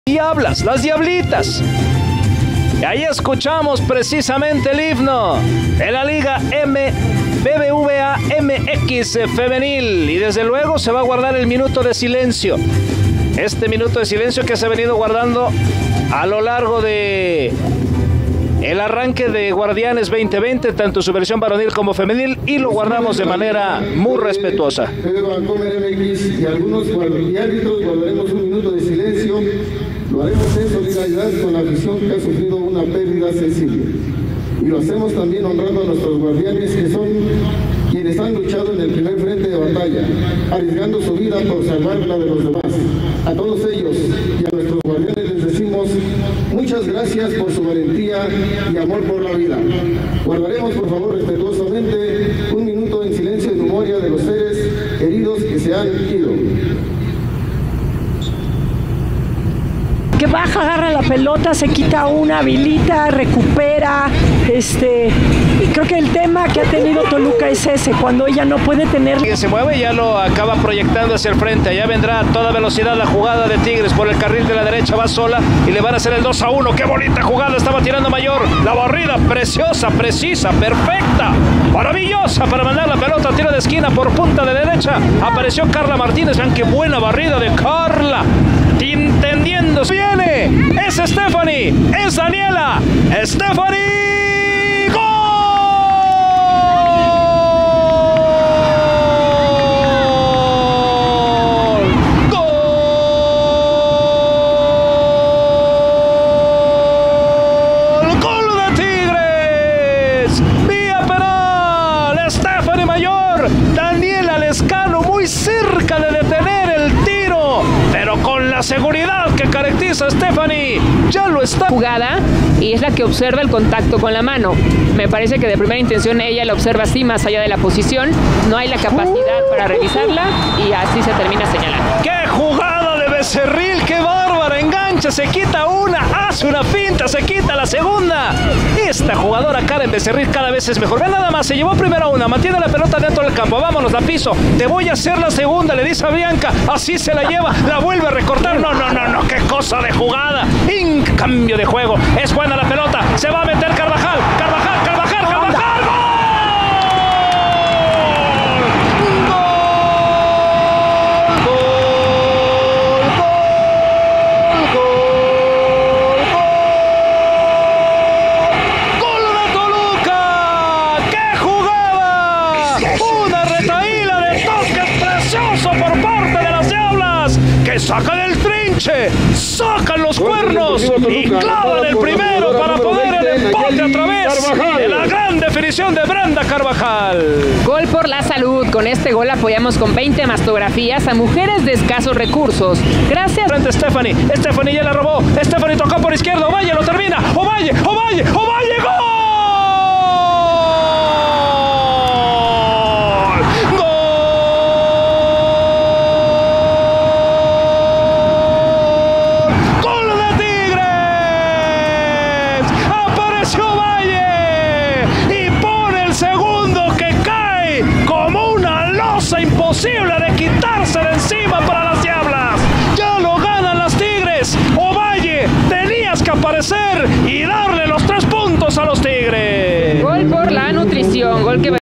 Resumen Toluca vs Tigres. Guard1anes 2020 LIGA BBVA MX Femenil
No te pierdas lo mejor del compromiso entre Toluca y Tigres de Guard1anes 2020 de la LIGA BBVA MX Femenil.